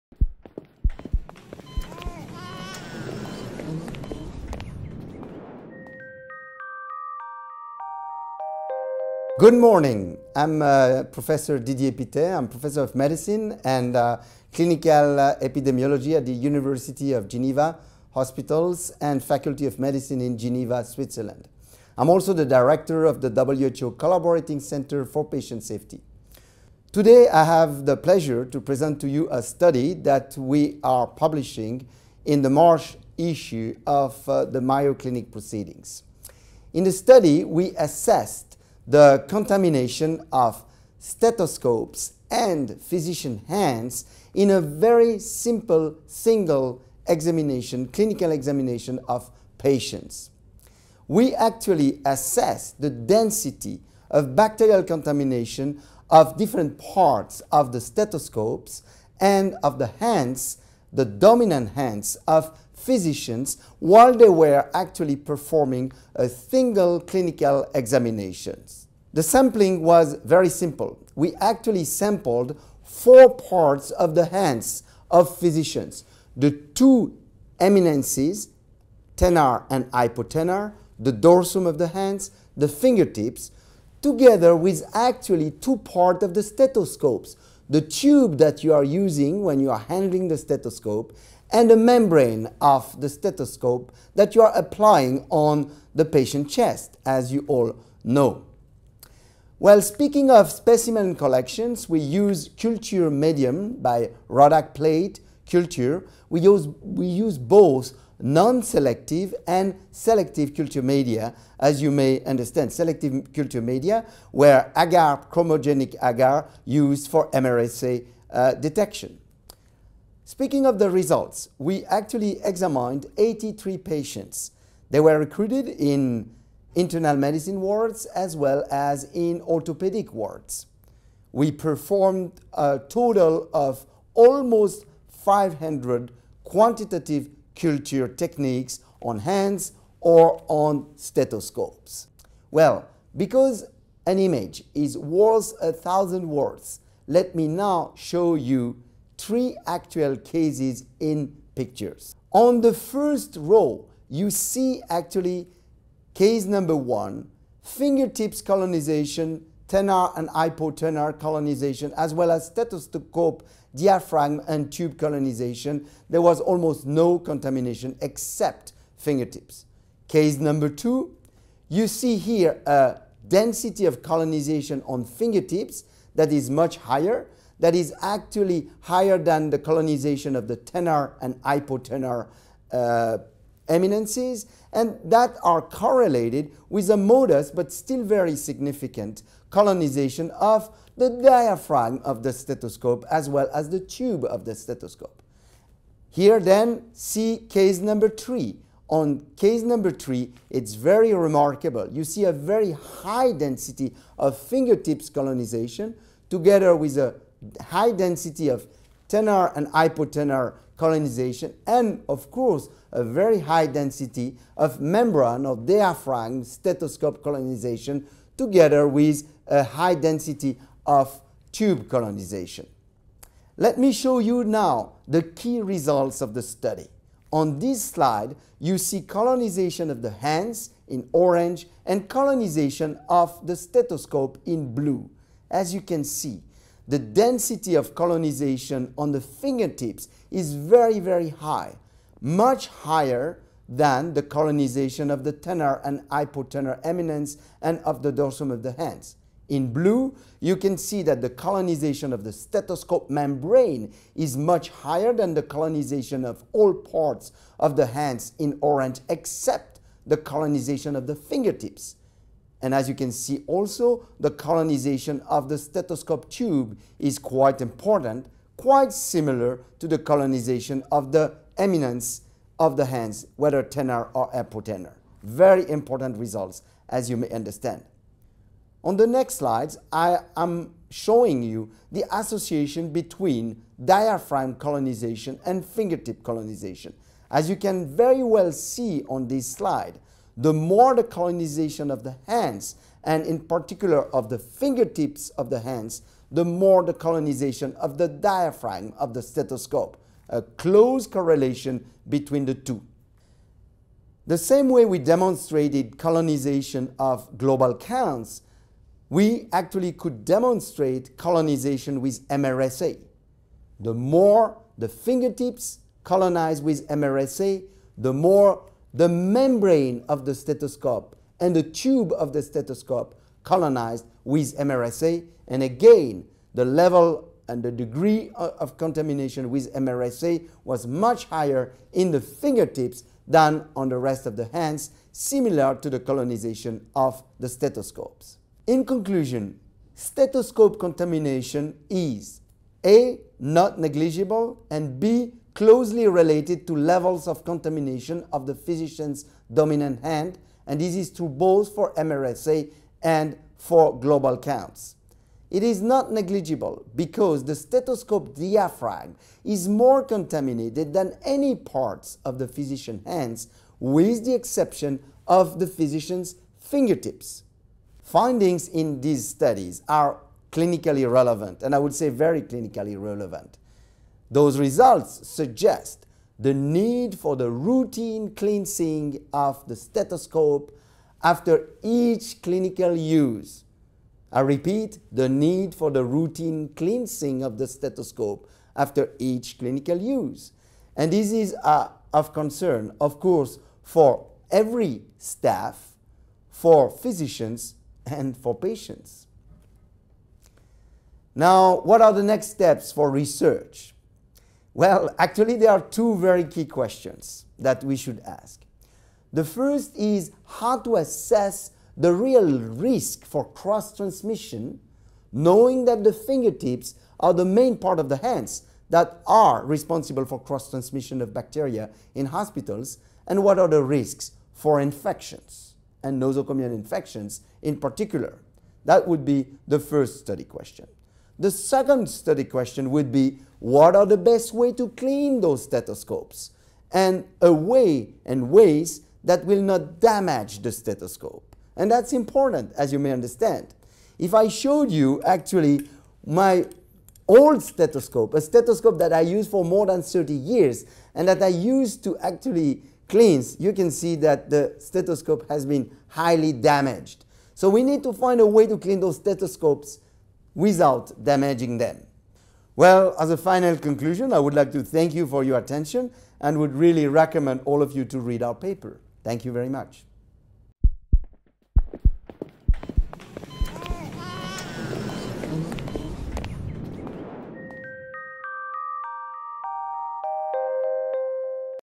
Contamination of stethoscopes and physicians' hands following a single physical examination
The study "Contamination of stethoscopes and physicians' hands following a single physical examination" is published in the March issue of Mayo Clinic Proceedings